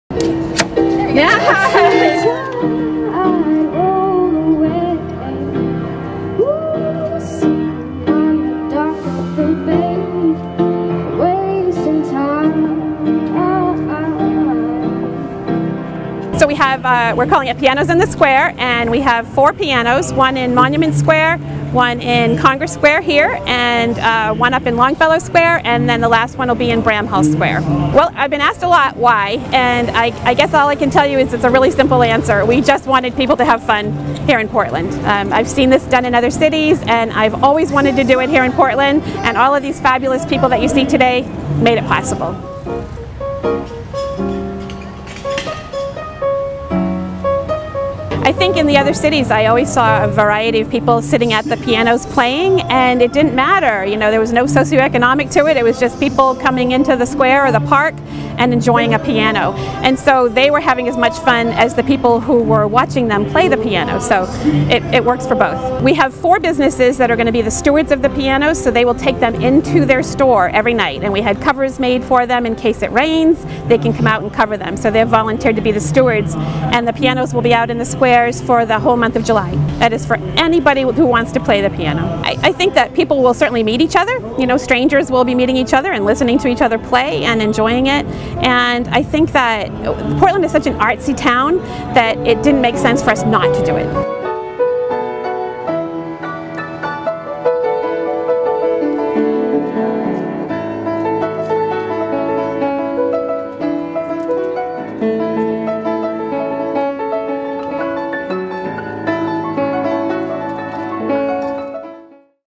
Pianos in the Squares
Squares along Congress Street rang with plinking keys and soaring voices on Friday as new public pianos were unveiled. The colorful instruments  painted by local artists  are part of the Pianos in the Squares Project and will be available for anyone to play during daylight hours through the end of July.